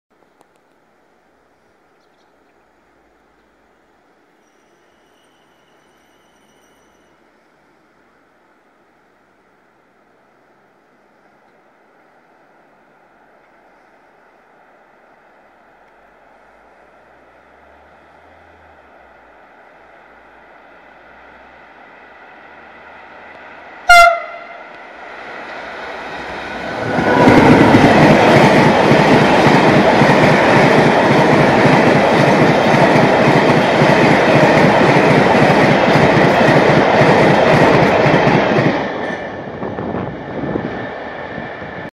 IL SALUTO DELLA TIGRE E.652 012 CON TRAMOGGE IN TRANSITO NELLA NEVE A ROBILANTE (CN) 8 - 12 - 2017
PROVENIENTE DA TRINO VERCELLESE CON FINE CORSA ALLO STABILIMENTO DELLA BUZZI UNICEM DI ROBILANTE.